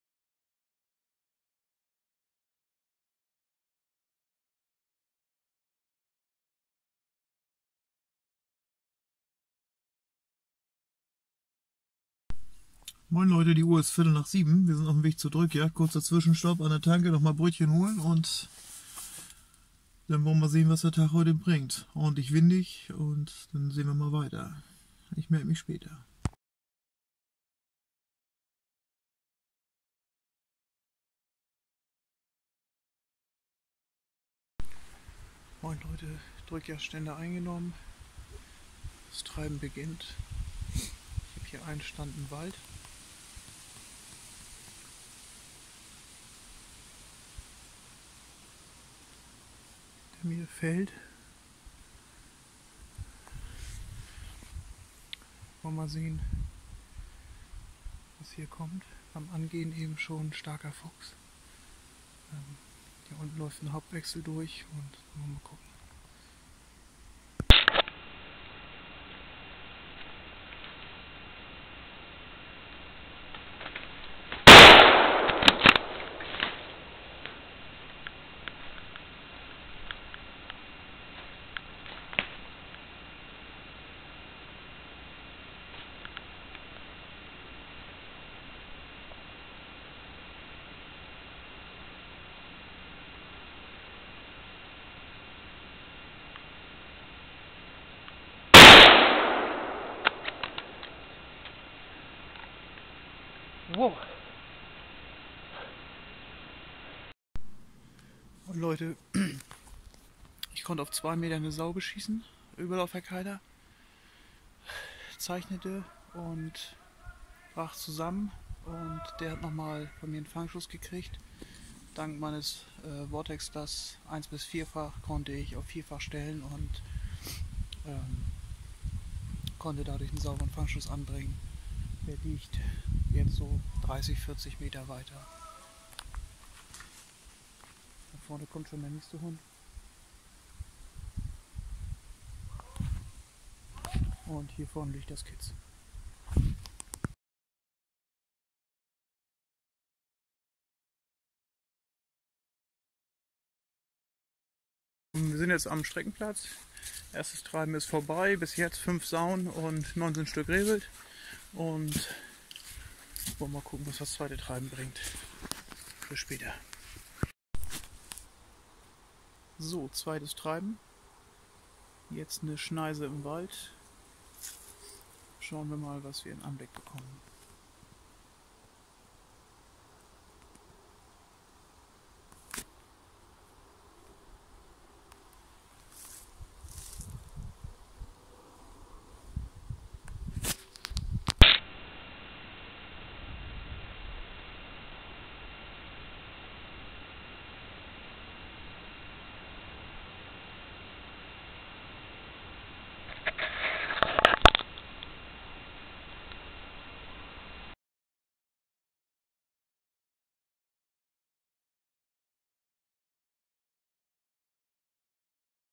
Drückjagd 2018
Drückjagd mit Freunden bei Freunden.
Tolle Jagd in einem Super Revier mit netten erfahrenen Leuten

Ein Überläuferkeiler von ca 80kg der sich vorher mit den Hunden in einem Brombeerverhau eine 3/4 Stunde gefetzt hatte, kam direkt auf mich zu und wird auf 2m von mir erlegt.
Zuvor konnte ich auf 30m ein Kitz strecken.